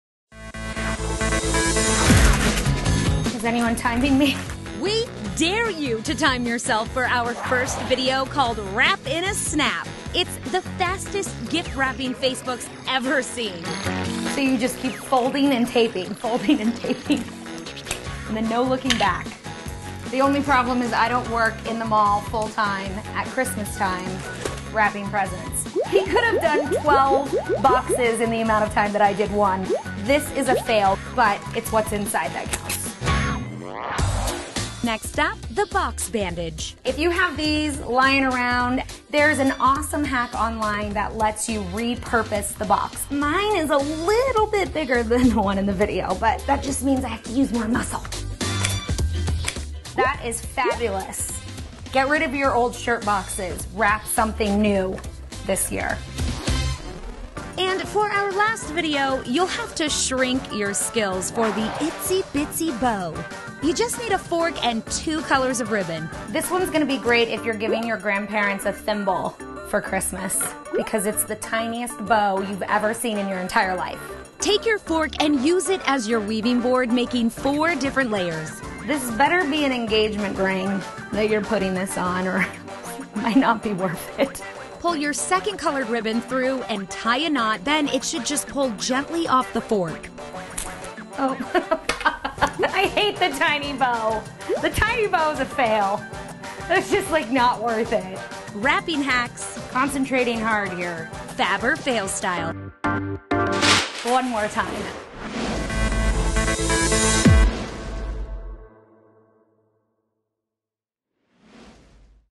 3 Festive Present Wrapping Hacks
Christmas is almost here, but if you haven’t wrapped your presents yet, never fear because The List’s Holly Morgan has vetted all those Facebook wrapping videos to find what Fab or Fail.

And online anytime at TheListTV.com!